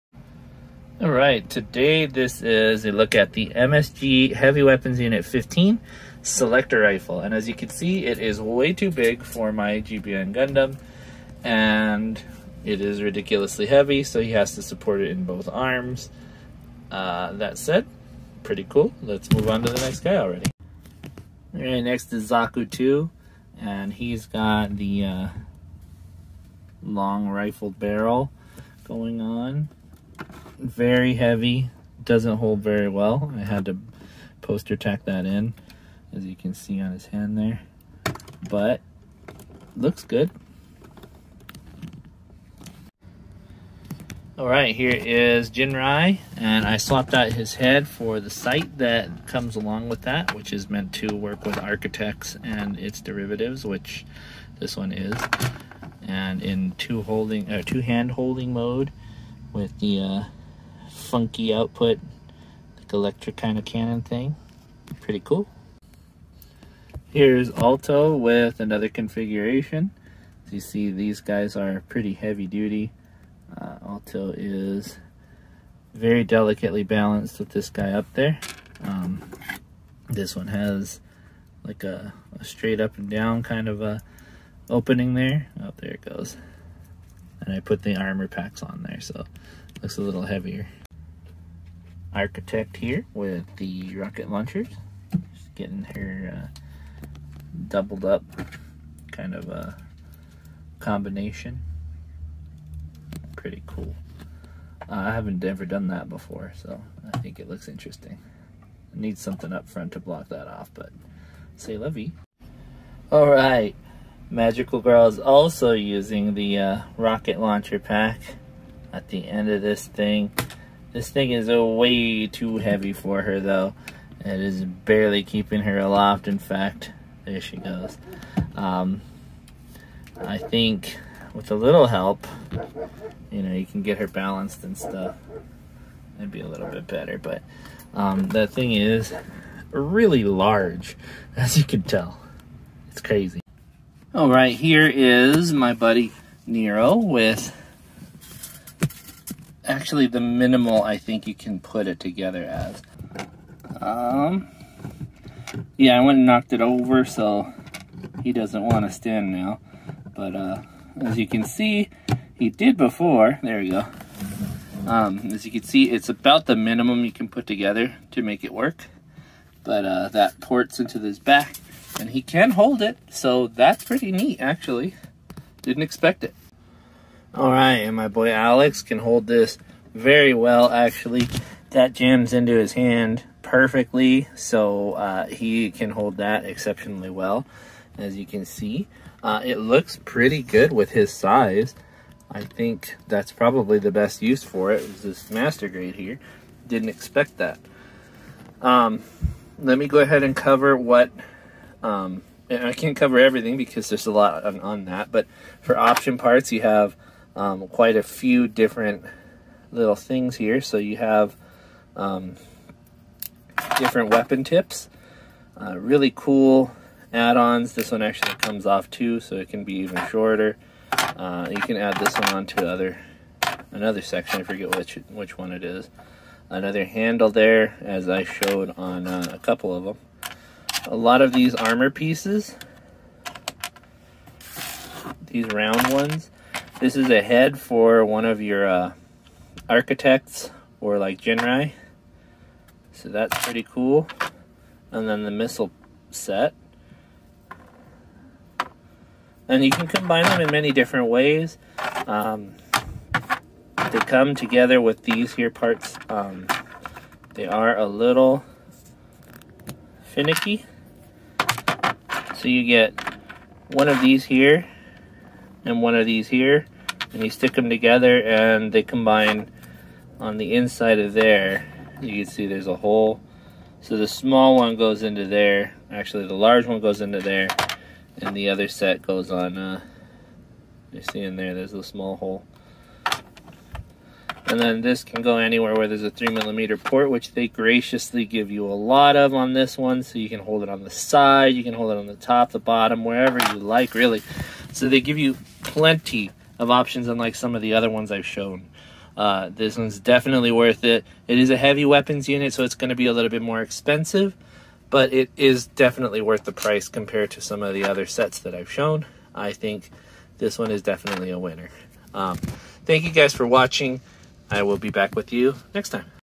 M.S.G. Heavy Weapon? Definitely Got that right. This Selector Rifle is HUGE
I know it's a Heavy Weapon Unit but Wow Is set 15 way too big!

Watch me desperately try to keep this thing aloft with several of my model kits, compare sizes with them and see what I can do with this HUMONGOUS rifle!